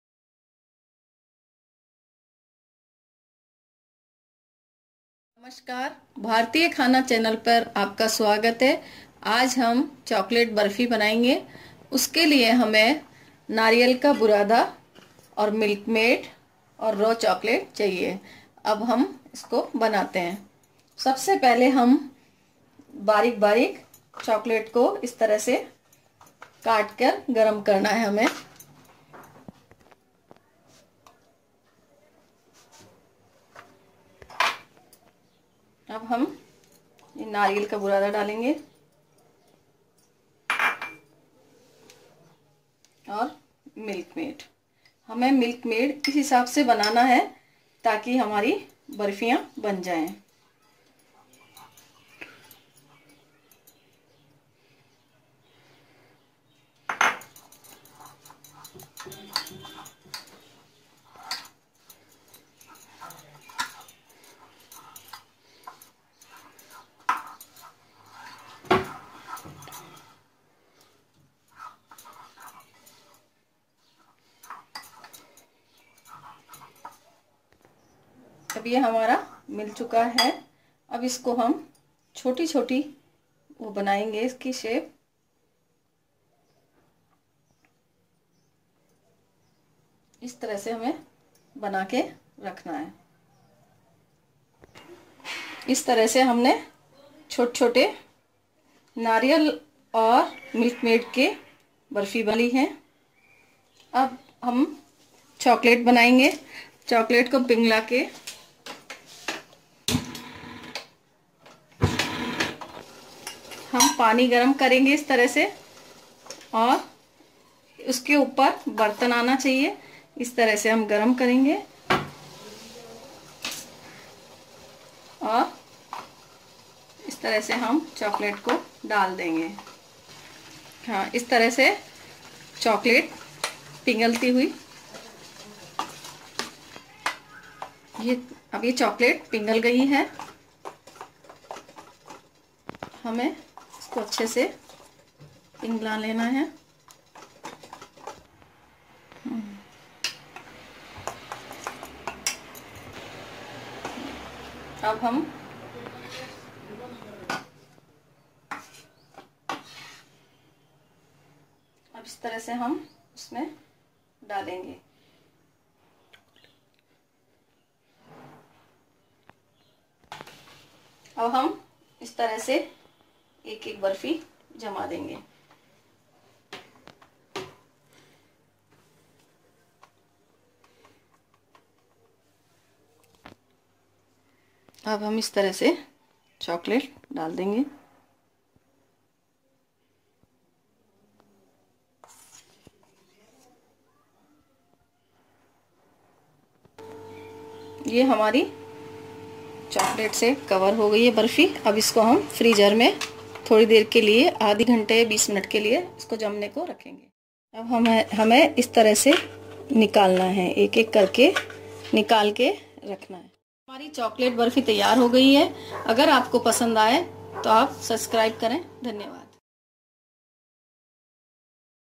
THE CHOCOLATE BARFI | 3 INGREDIENTS RECIPE | CHOCOLATE BARS.... 2016 DIWALI SPECIAL
CHOCOLATE BARFI OR CHOCOLATE BARS
ITS EASY AND SIMPLE SWEET DISH
HOMEMADE RECIPE ESCPECIALLY FOR CHILDREN

INGREDIENTS ARE
COCONUT POWDER
MILKMAID
RAW CHOCOLATE

AS YOU CAN SEE WE REQUIRE ONLY 3 INGREDIENTS
CHECK THIS EASY RECIPE
SHARE YOUR VIEWS WITH US

CHECK US OUT
THANKYOU